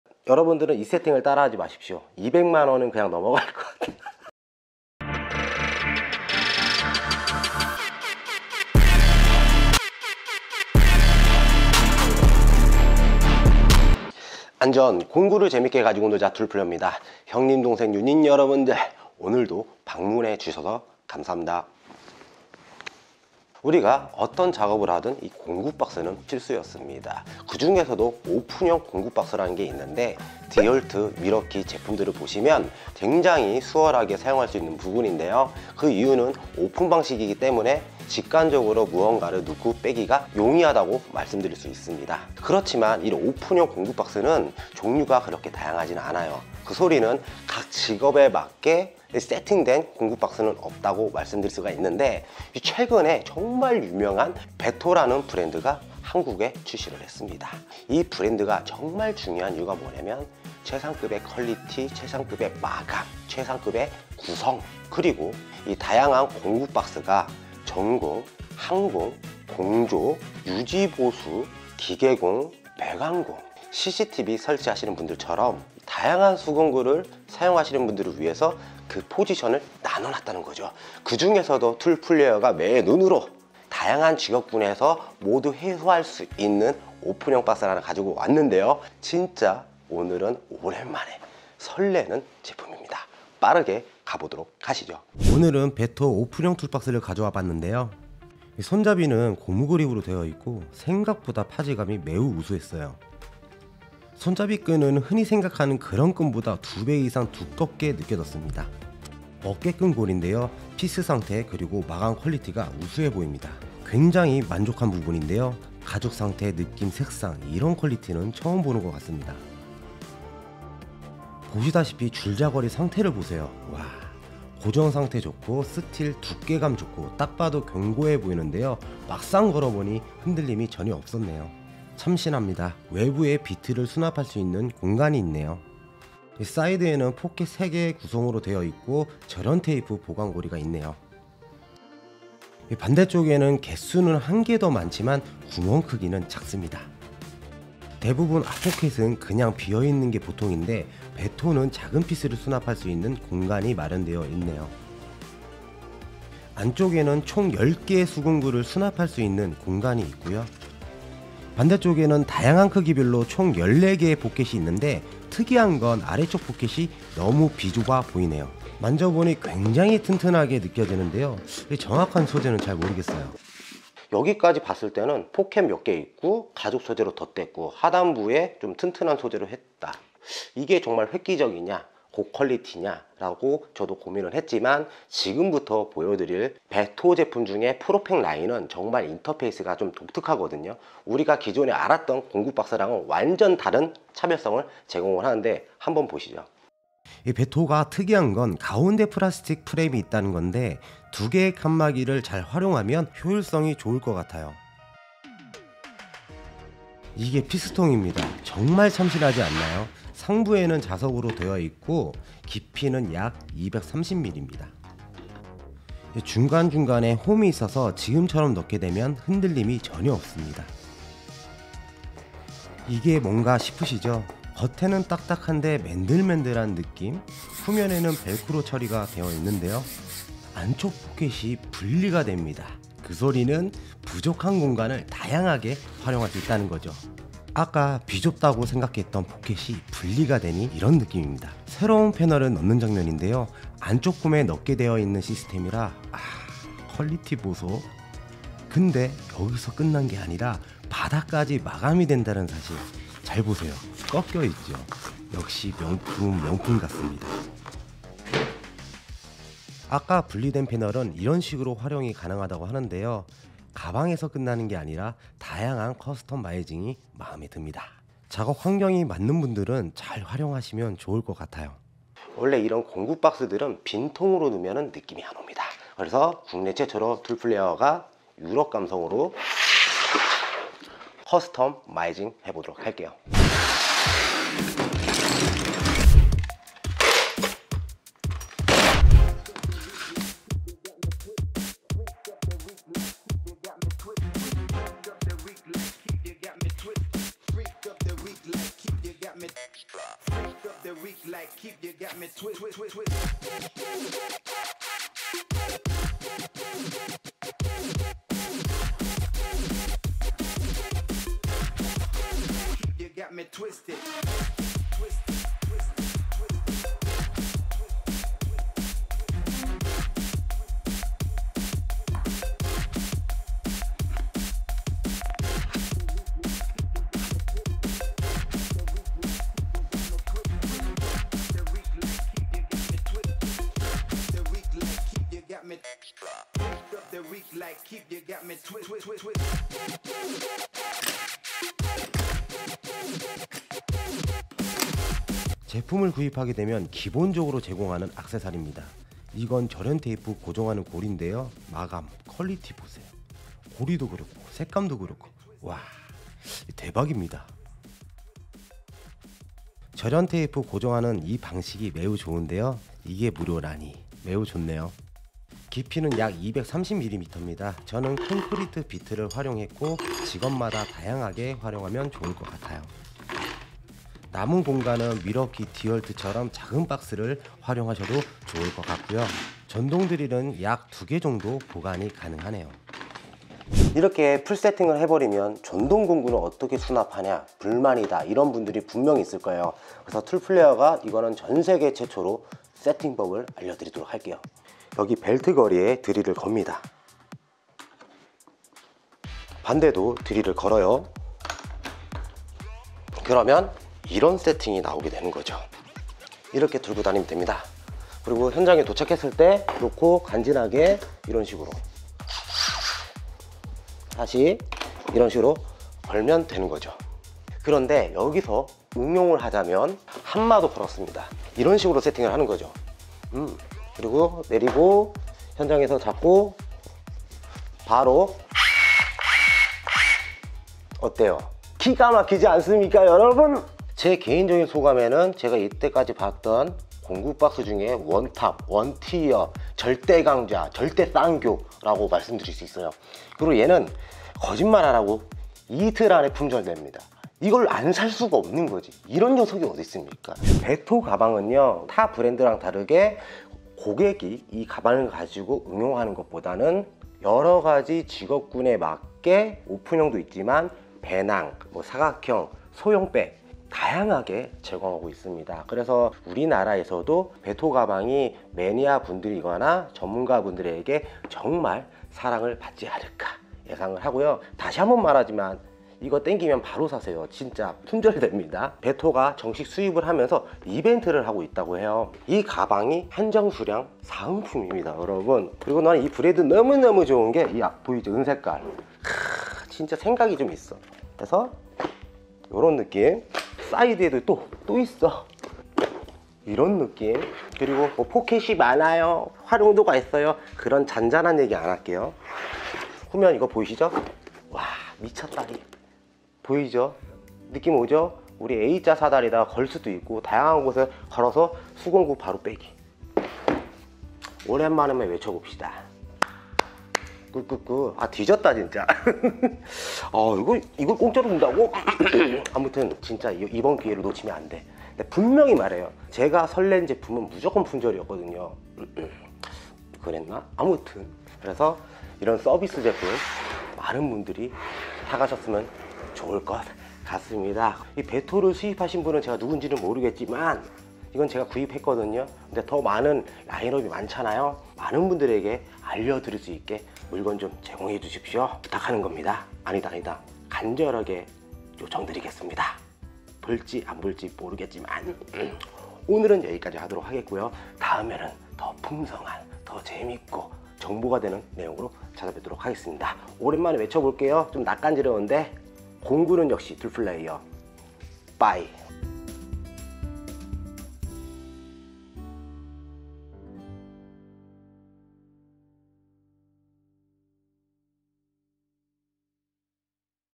공구 가방도 명품이 있다?! 직업별로 커스텀 마이징이 가능한 세계 유일 끝판왕 한국 출시 (베토 TECH OT-LC)
필요하시다면 빨리 사세요.. 품절돼요...
제품 스펙이 궁금하다면?